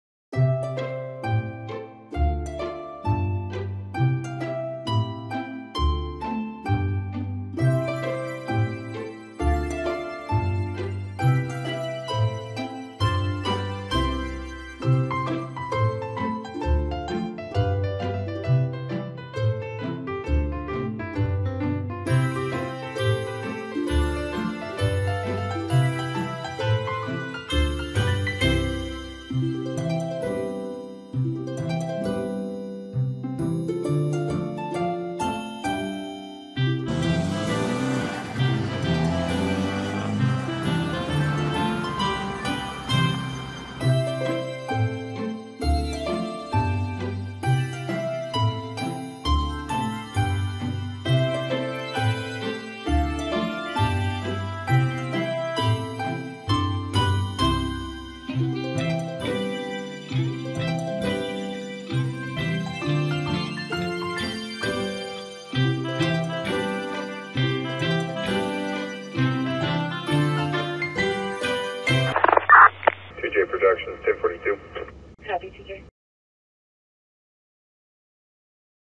Provo PD - Crossing Guard Promo - Provo City Police
Provo City is needing assistance with crossing guards.  Apply today at ProvoCity.jobs